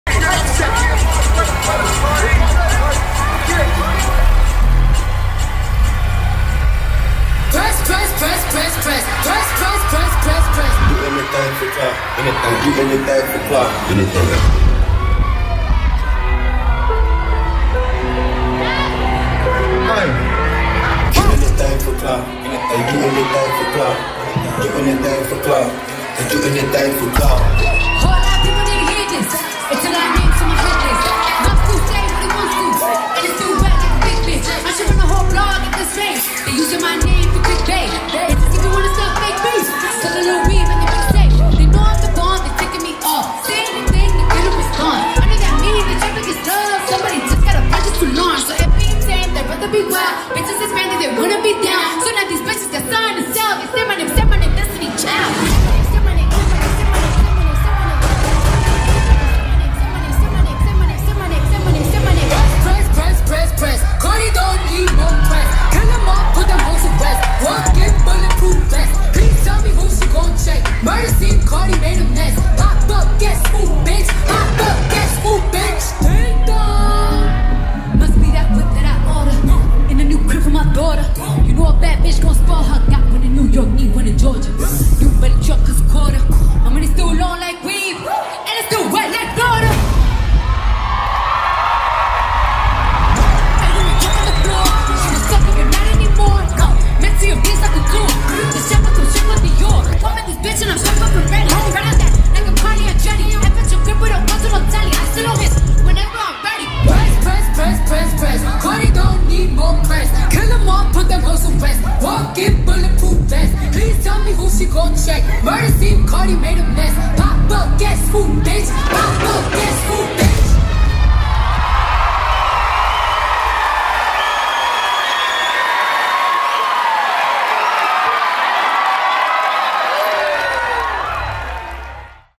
SZN 12 | IN CONCERT | ADVANCED GROUP | JAC VALIQUETTE CHOREOGRAPHY
This is a NEW video from our recent Badass Babes showcase! Our dancers train intensively with us in different levels of programs, and work hard on choreography and technique to put on a production at the end of their season!

Video by @dannydavalos_